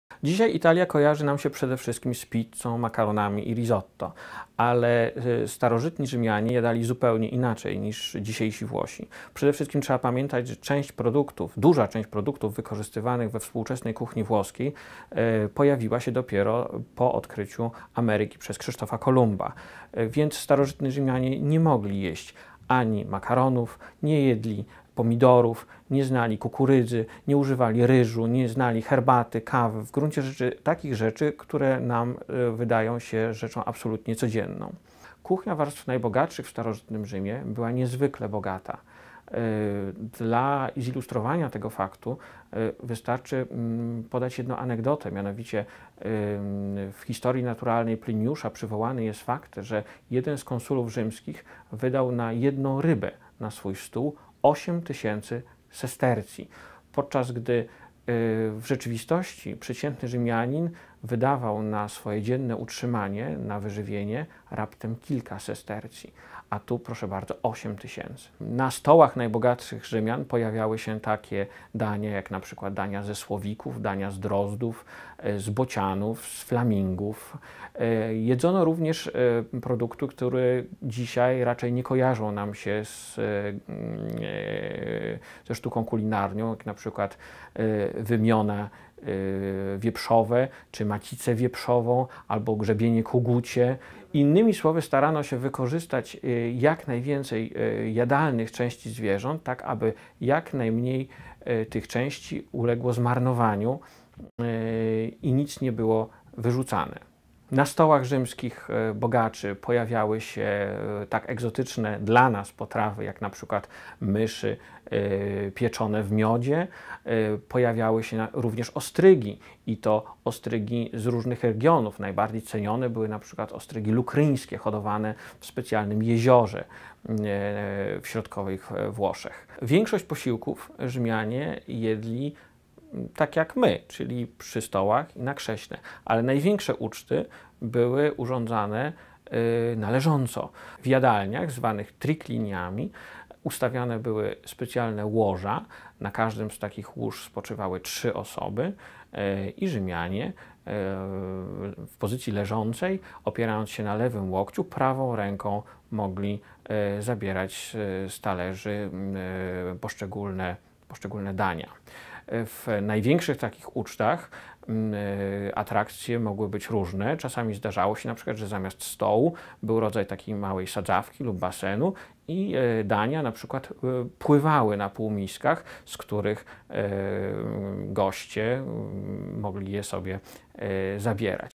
Mysz w miodzie to dawny przysmak rzymian
Wypowiedź: Michał Kubicz, autor książki „Agrypina. Cesarstwo we krwi”.

Italia kojarzy nam się z pizzą, makaronami i risotto, ale starożytni Rzymianie jadali zupełnie inaczej niż dzisiejsi Włosi. Powód? Duża część produktów wykorzystywanych we współczesnej kuchni włoskiej pojawiła się dopiero po odkryciu Ameryki przez Krzysztofa Kolumba. Starożytni Rzymianie nie mogli jeść ani makaronów, nie jedli pomidorów, nie znali kukurydzy, nie używali ryżu, herbaty i kawy. Co zatem jadali starożytni Rzymianie?

- Na stołach najbogatszych Rzymian pojawiały się takie potrawy, jak np. dania ze słowików, z drozdów, bocianów i flamingów. Jedzono również produkty, które dzisiaj raczej nie kojarzą nam się ze sztuką kulinarną np. wymiona i macice wieprzowe, albo grzebienie kogucie. Starano się wykorzystać jak najwięcej jadalnych części zwierząt, żeby jak najmniej uległo zmarnowaniu i nic nie było wyrzucane – mówi newsrm.tv Michał Kubicz, autor książki „Agrypina. Cesarstwo we krwi”.

Na tym jednak nie koniec. Na stołach rzymskich bogaczy pojawiały się tak egzotyczne dla nas potrawy jak na przykład myszy pieczone w miodzie. Jadano także ostrygi. Najbardziej cenione był ostrygi lukryńskie hodowane w specjalnym jeziorze w środkowych Włoszech.